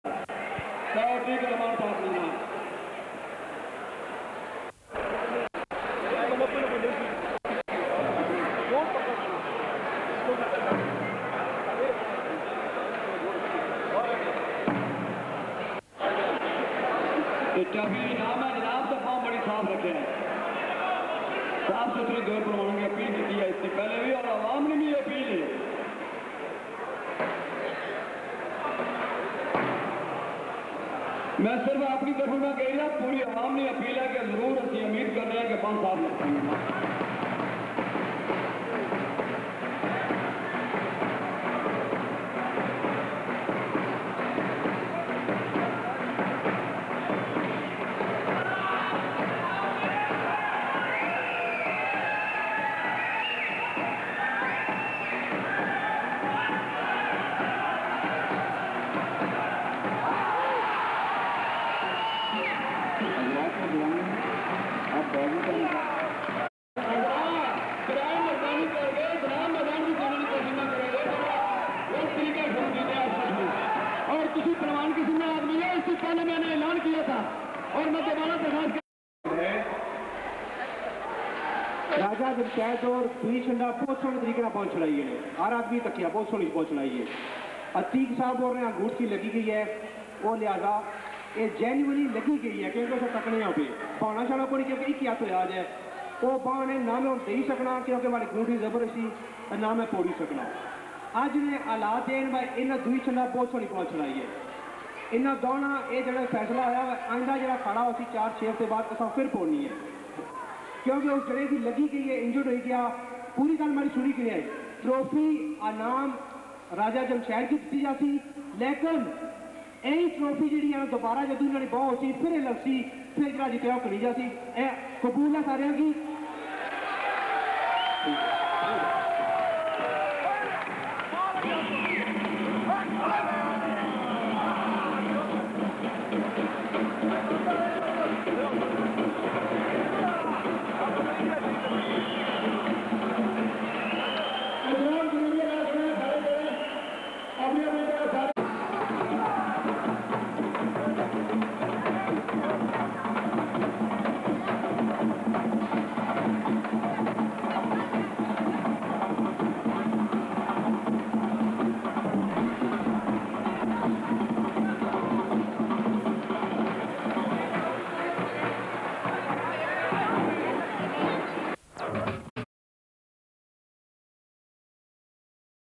Bini - Pehlwan Ch Atiq vs Pehlwan Raja Jamshed
Bini Between Two Great Heavy Weight Legend Pehalwan Ch Atiq From Bradford Vs Pehalwan Raja Jamshed.
Ch Atiq Injured His Thumb During This  Bini Match and Rematch Was Within 3 to 4 Month Time.